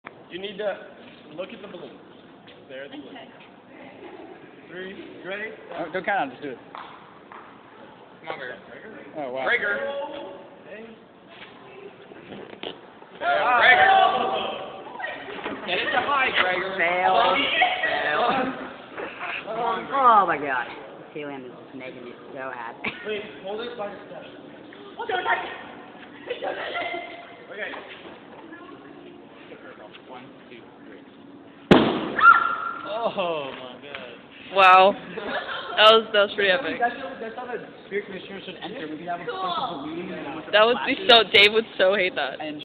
Flash Bulb Balloon Burst
Gregor bursts a balloon with flash bulb.